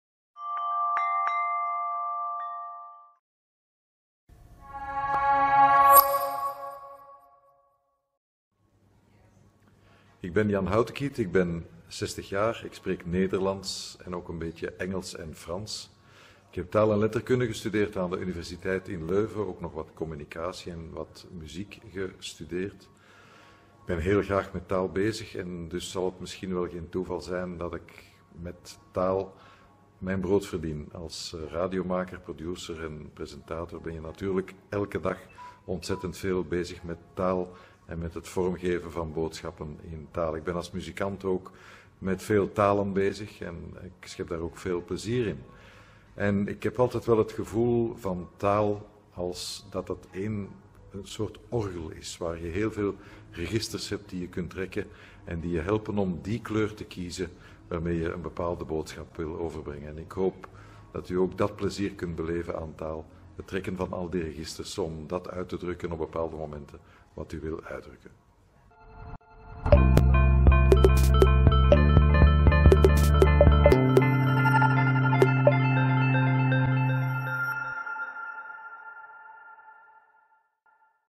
Taalvertellers: Jan Hautekiet
Radiopresentator, muzikant en taalverteller Jan Hautekiet stelt zichzelf voor. Lees het interview op